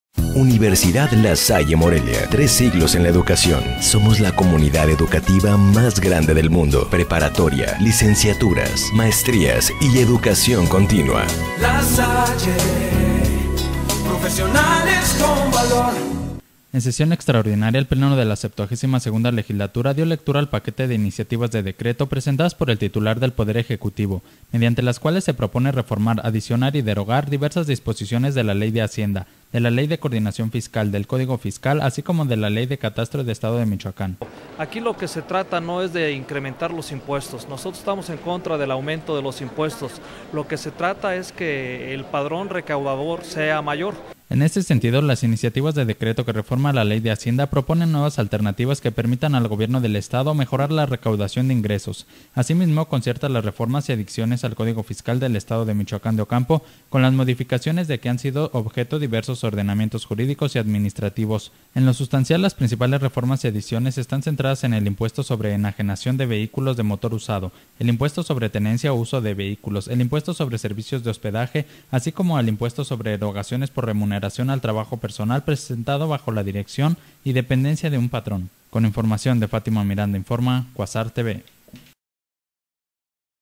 Analizan diputados propuestas del ejecutivo para mejorar recaudación fiscal
En sesión extraordinaria, el pleno de la 72 Legislatura, dio lectura al paquete de iniciativas de decreto presentadas por el titular del Poder Ejecutivo, mediante las cuales se propone reformar, adicionar y derogar diversas disposiciones de la Ley de Hacienda; de la Ley de Coordinación Fiscal; del Código Fiscal; así como de la Ley de Catastro del Estado de Michoacán…